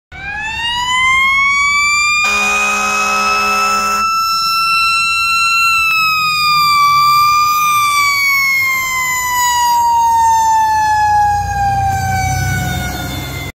Cary Engine 2 Responding to Fire Alarm | 9-21-2022 | Cary, NC
Cary Engine 2 responding to a fire alarm at 8:55 PM on September 21st, 2022. Cary Engine 2 was operating on a reserve apparatus at the time.
The intercept was made a few blocks from Cary Station 2, at the cross street of Maynard Road and East Chatham Street.